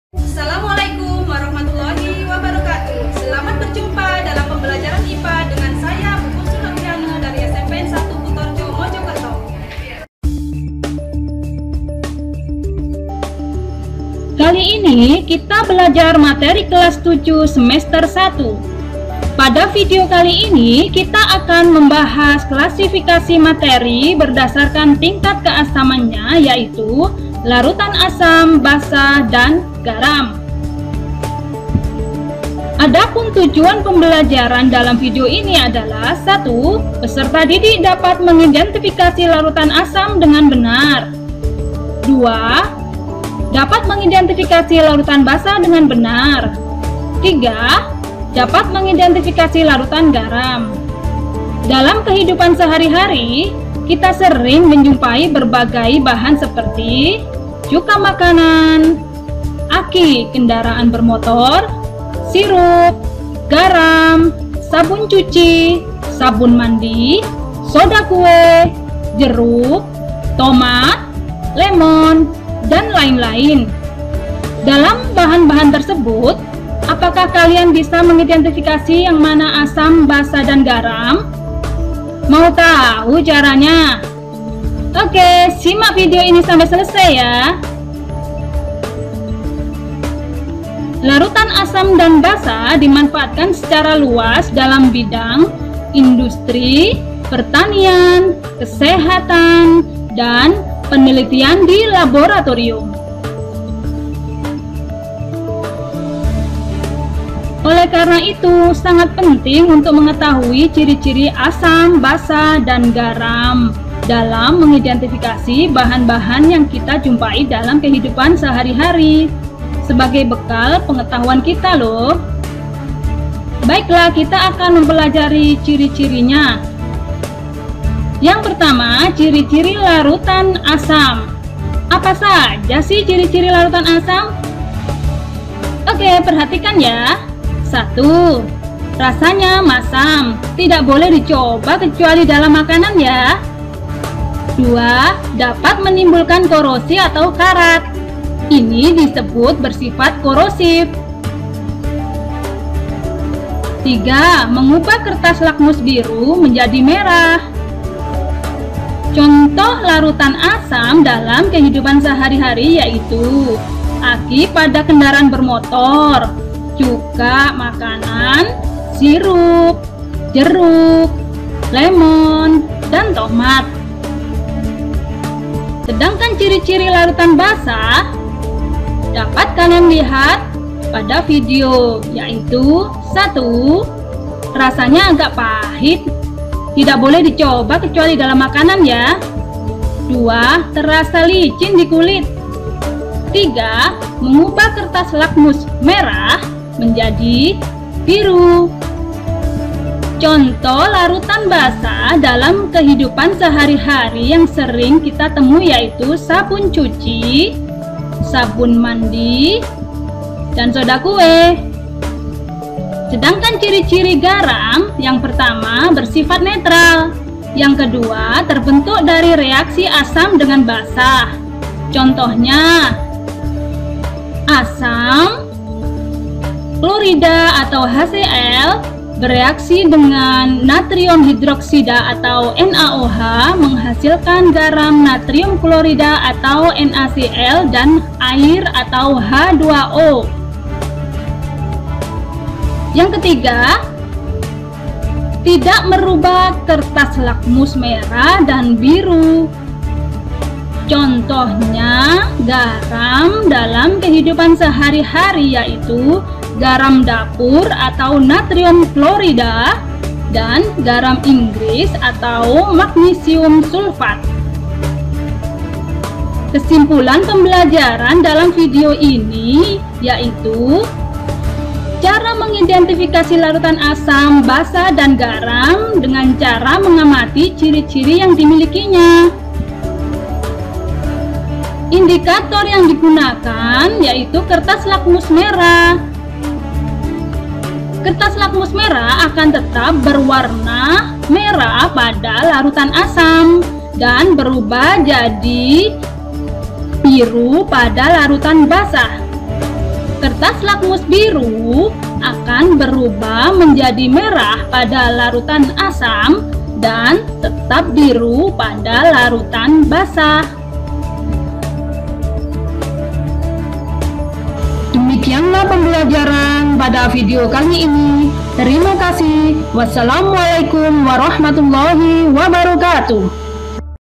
LARUTAN ASAM, BASA, DAN GARAM KELAS VII SEMESTER 1
nama : CHUSNUL ARFIANE, M.Pd.
Instansi : SMPN 1 KUTOREJO

Larutan asam, basa, dan garam kelas VII semester 1 Kurikulum 2013 dari SMPN 1 Kutorejo Mojokerto